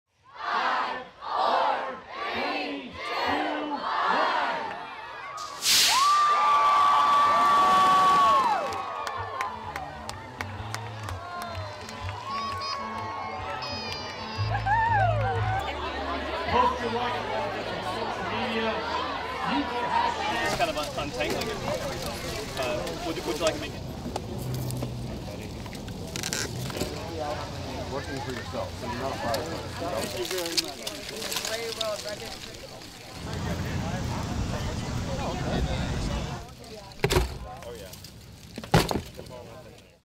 پرتاب هزاران آپولو ۱۱ کوچک در سالگرد اعزام فضاپیما به ماه
روز سه‌شنبه ۱۶ ژوئیه مراسم پرتاب هزاران موشک کوچک مشابه آپولو ۱۱ همزمان با پنجاهمین سالگرد پرتاب این فضاپیما به ماه در شهر هانتسویل آلاباما در آمریکا برگزار شد.…